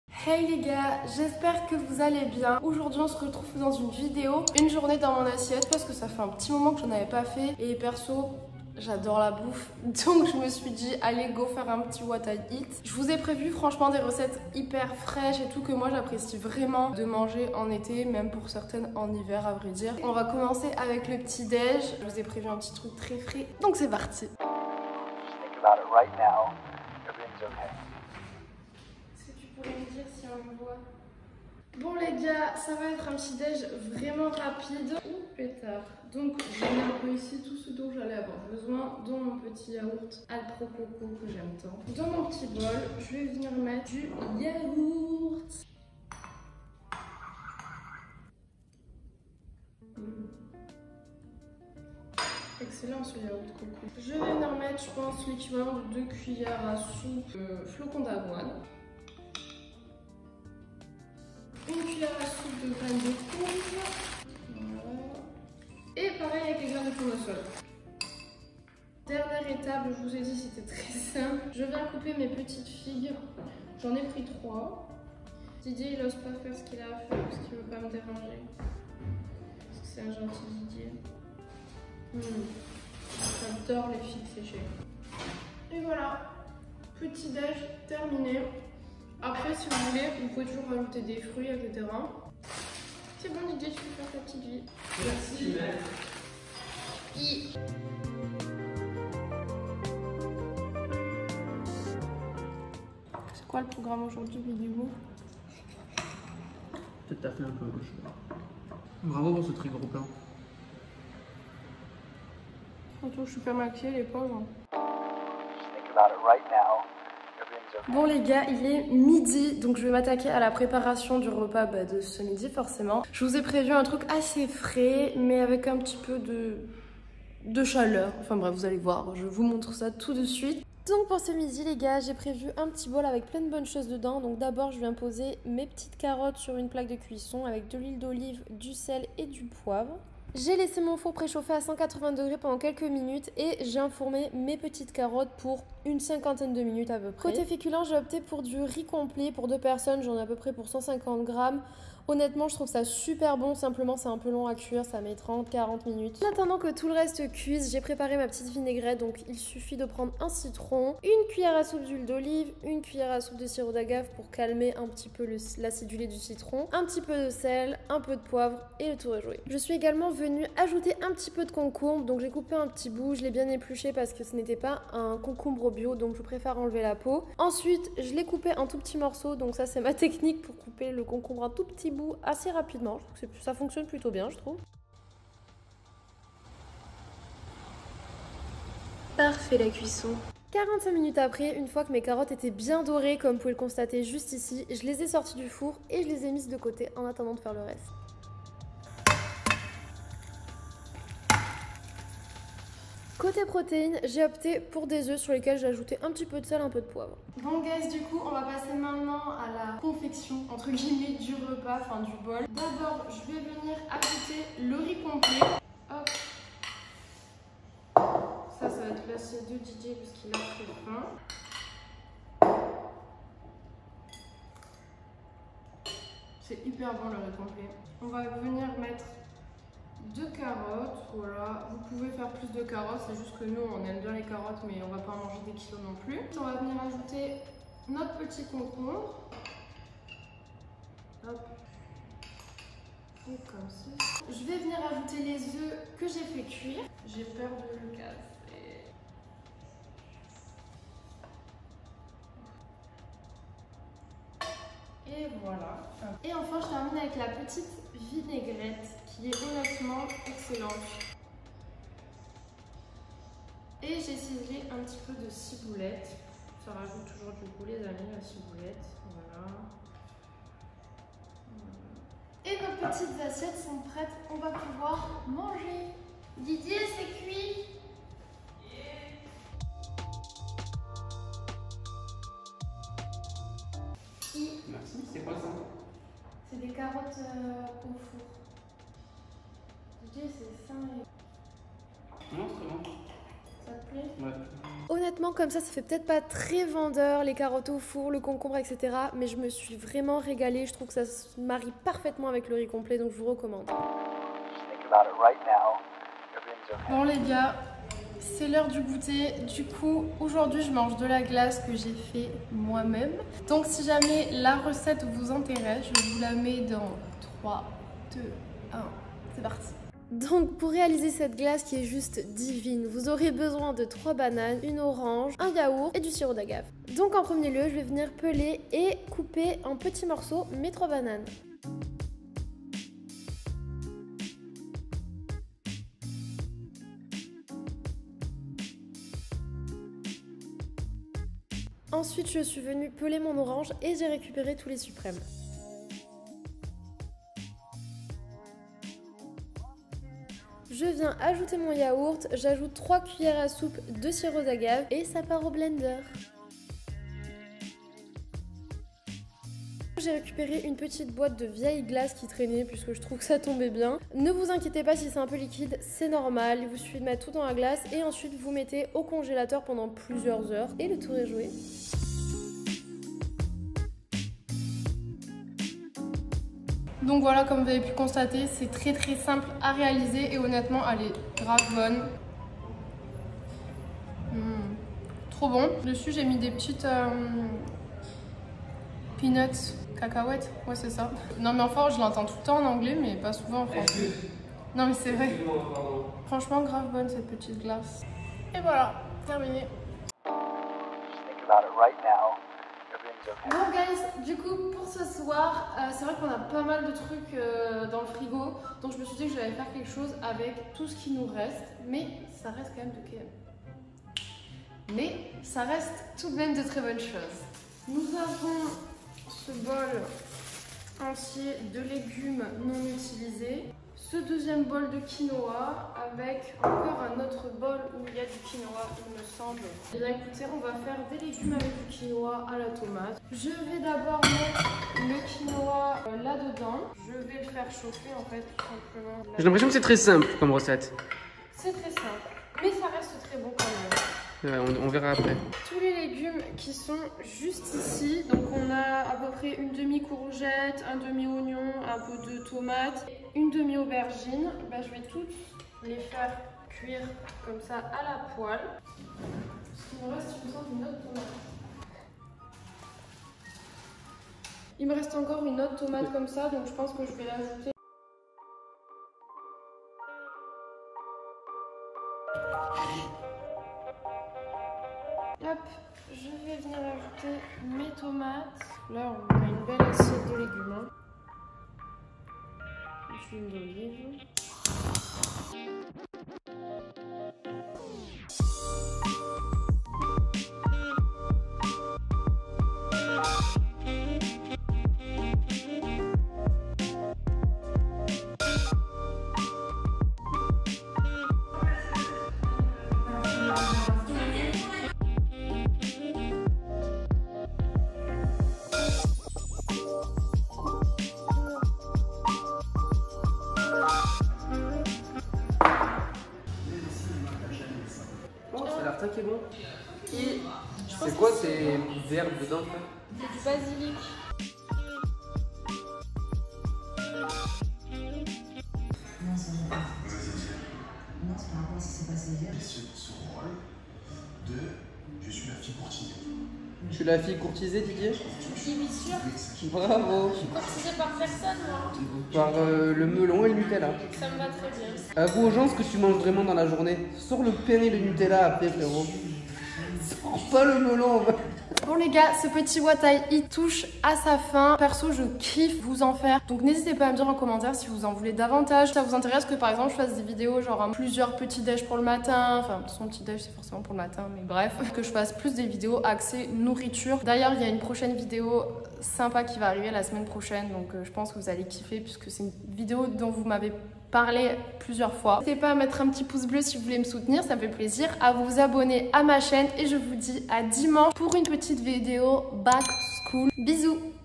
UNE JOURNÉE DANS MON ASSIETTE - Ventre plat et tonique
Heyyy,
On passe une journée ensemble. Je vous présente les différents repas que j'aime manger !
En espérant que celle ci vous plaira ! 🥰